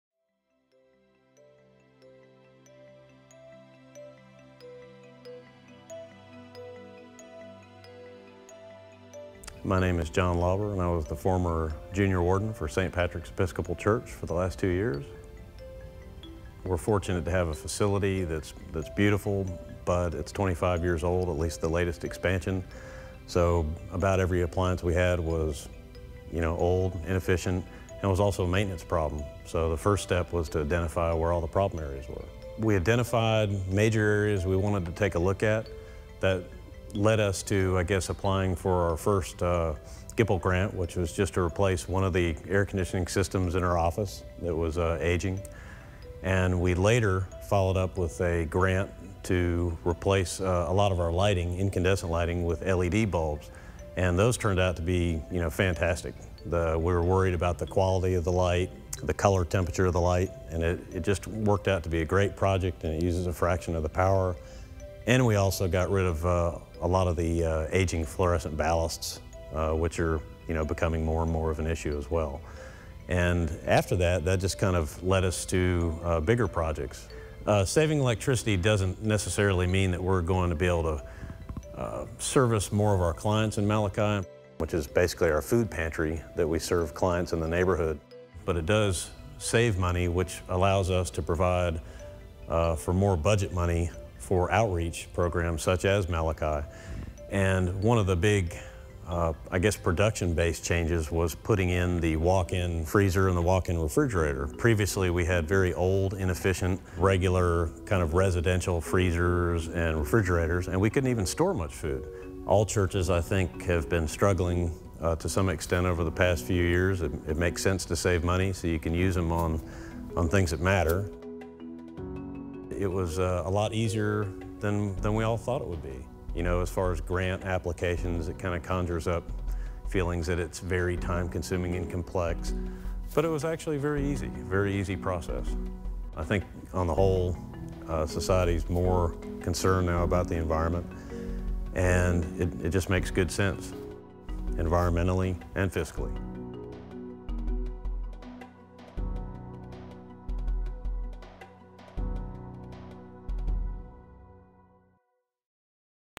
Success Story: St. Patrick's Episcopal Church
- John Lauber of St. Patrick's Episcopal Church in Dunwoody talks about upgrading their inefficient building with the help of GIPL's Matching Grants.